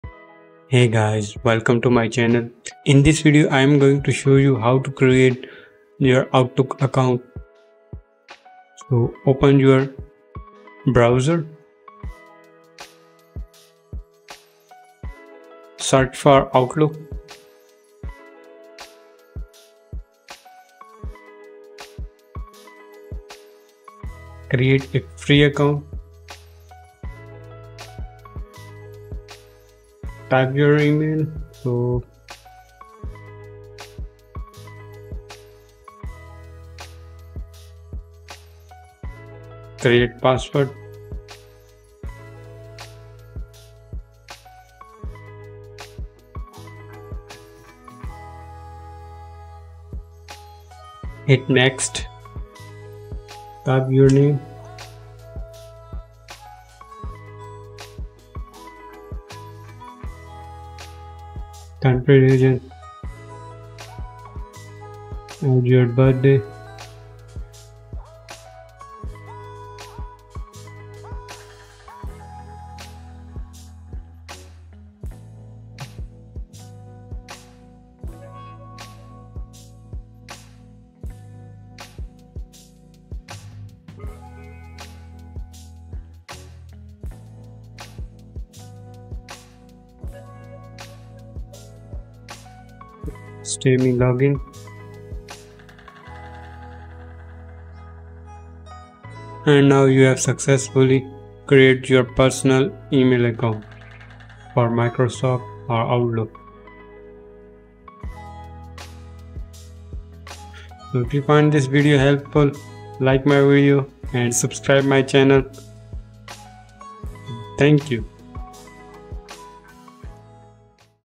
How to Create a Microsoft Outlook Profile (A Step-by-Step Guide)
How to Create a Microsoft Outlook Profile (A Step-by-Step Guide).In this tutorial, you will learn how to create a Microsoft outlook profile.